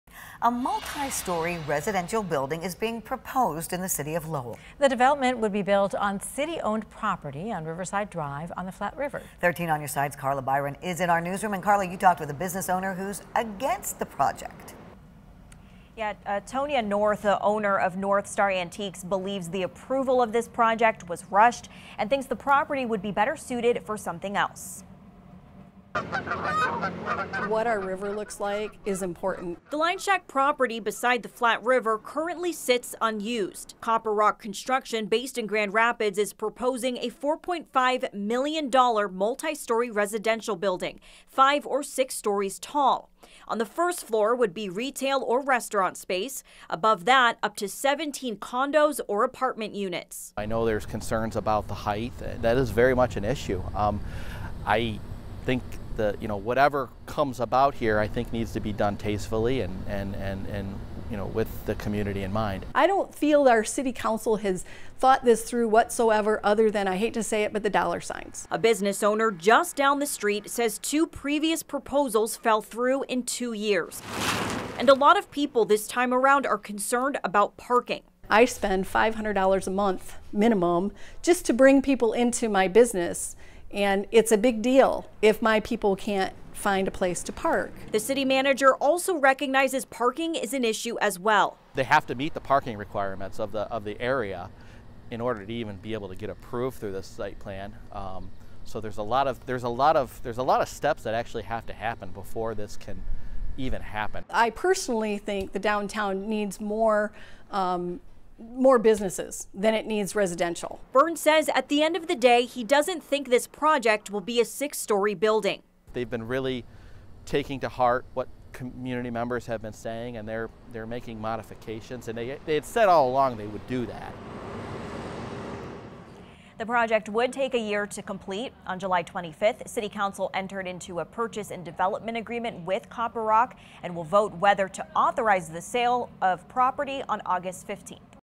Proposed multi-story residential building in Lowell worries nearby businesses
On July 25, City Council entered into a purchase and development agreement with CopperRock Construction.